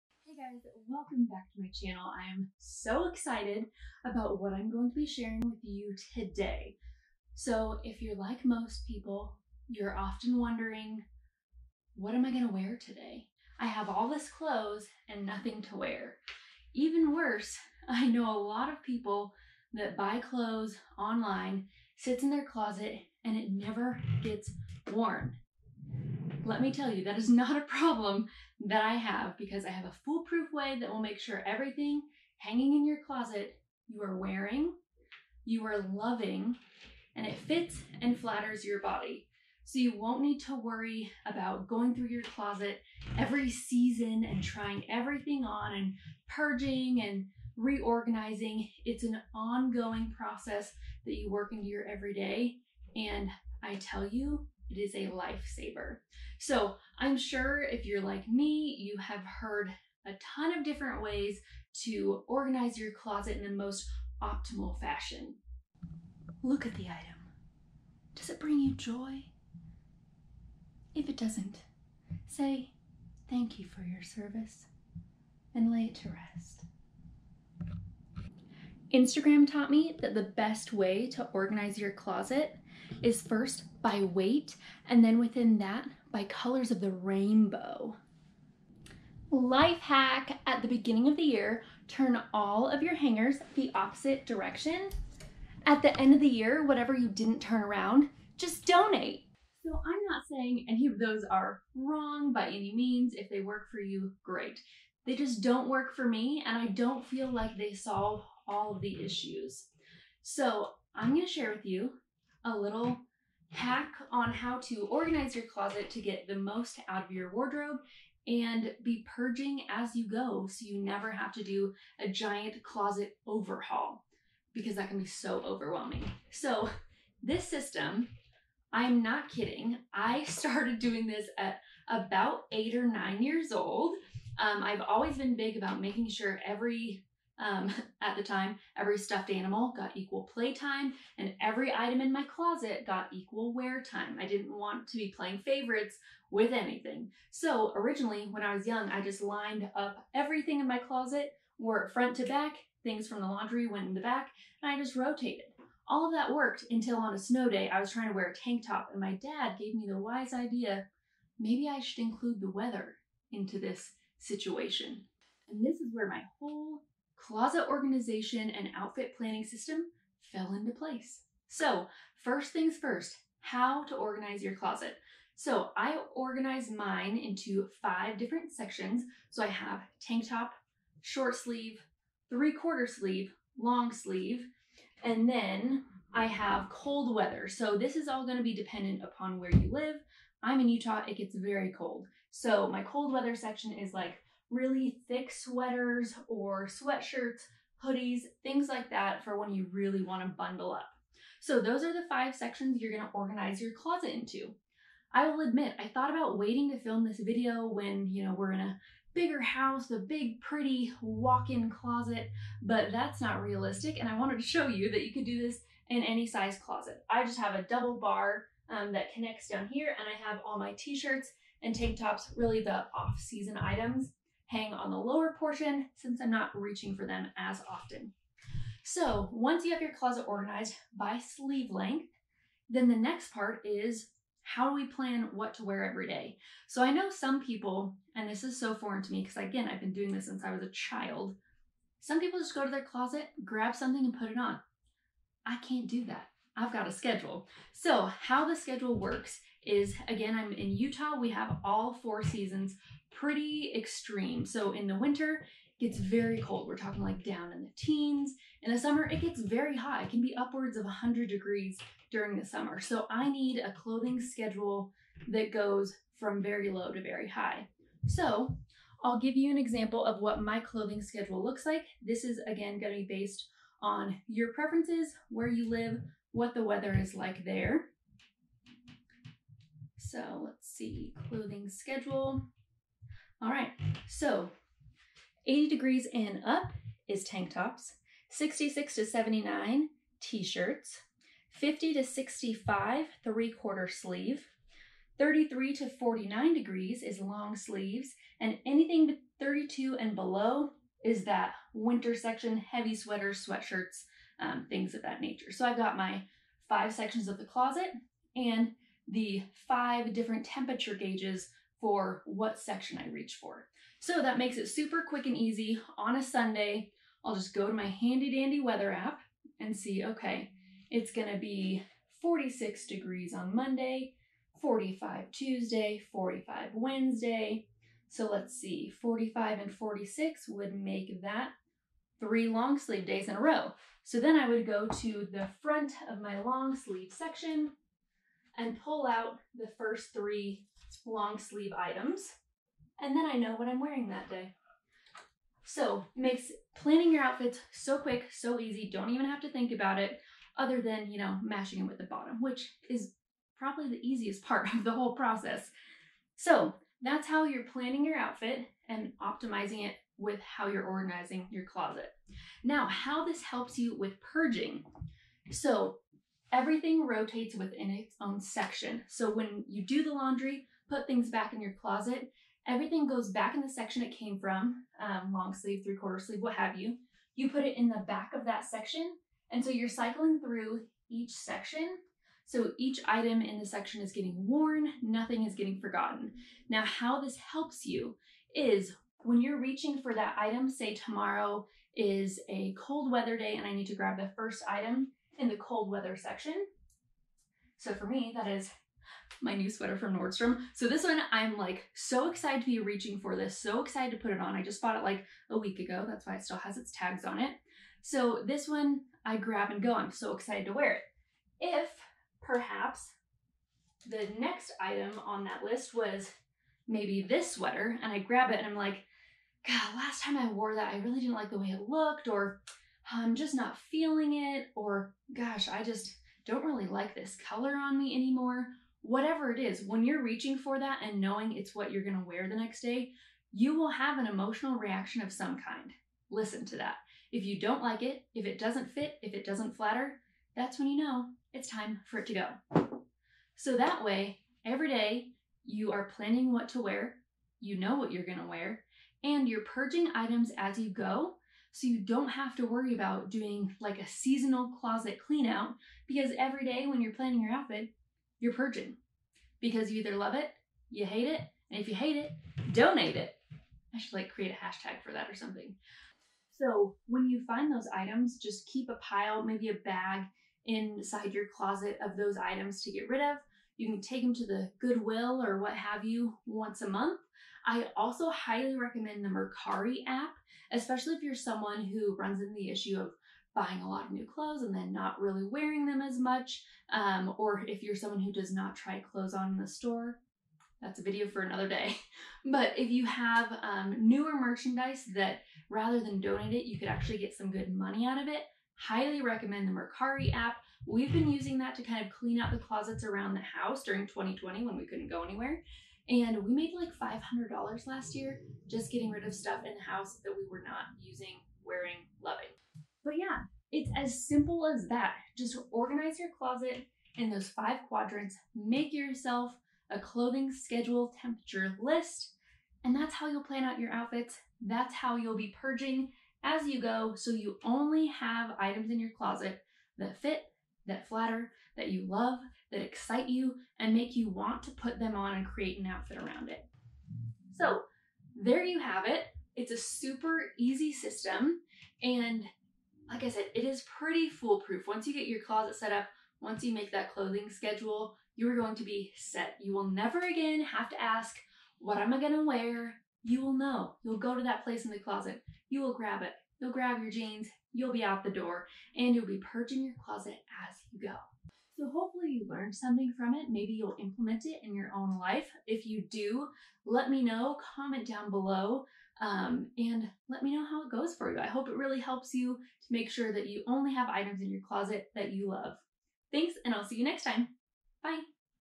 Nothing to wear? Easy Outfit Planning & Closet Organization Guide
Plan your outfits in minutes and optimize your wardrobe organization with my easy and foolproof system! Organize your closet, plan your outfits, and purge as you go so you never had to waste a whole day on an "overhaul." It's a tried and true system you can build into your life to ensure your closet is full of pieces that fit, flatter, and fill you with joy!